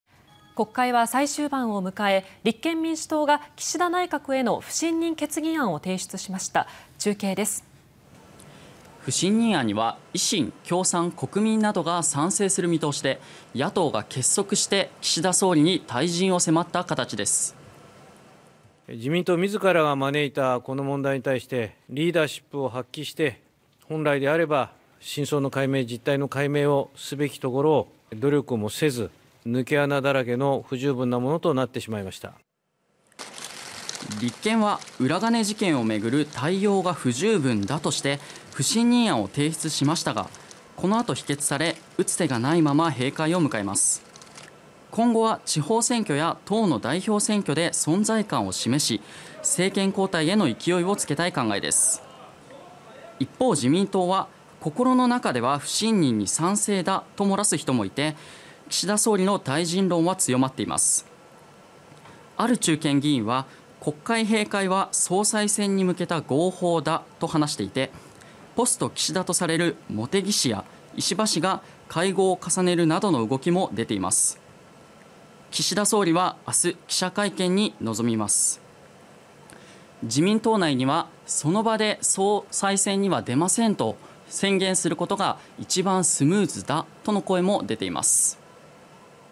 国会閉会へ…「総裁選の号砲」 内閣不信任案は午後否決(2024年6月20日)
国会は最終盤を迎え、立憲民主党が岸田内閣への不信任決議案を提出しました。

　（政治部・平井聡一郎記者報告）
　不信任案には、維新、共産、国民などが賛成する見通しで、野党が結束して岸田総理に退陣を迫った形です。

立憲民主党　安住国対委員長
「自民党自らが招いたこの問題に対して、リーダーシップを発揮して、本来であれば真相の解明、実態の解明をすべきところ、努力もせず、（法改正は）抜け穴だらけの不十分なものとなってしまいました」

　立憲は、裏金事件を巡る対応が不十分だとして、不信任案を提出しましたが、このあと否決され、打つ手がないまま閉会を迎えます。

　今後は地方選挙や党の代表選挙で存在感を示し、政権交代への勢いをつけたい考えです。

　一方、自民党は、心の中では不信任に賛成だと漏らす人もいて岸田総理の退陣論は強まっています。

　ある中堅議員は、国会閉会は「総裁選に向けた号砲だ」と話していて、ポスト岸田とされる茂木氏や石破氏が会合を重ねるなどの動きも出ています。

　岸田総理は21日、記者会見に臨みます。

　自民党内からは「その場で総裁選には出ませんと宣言することが一番スムーズだ」との声も出ています。